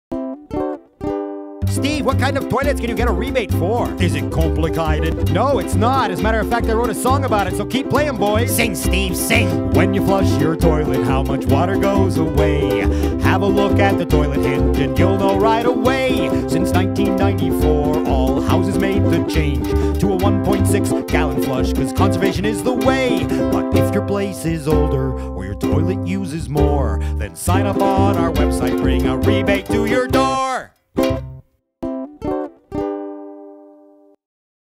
David Cassel | Plumber Steve 04 | Broward Water Partnership
This is episode 4 in my series of 7 short ad spots for the Broward Water Partnership Conservation Pays Water Rebate campaign. Featuring Plumber Steve and his band the Tub Boys, Ernesto on Ukulele, Phil on drums and Sid, from Australia, on bass.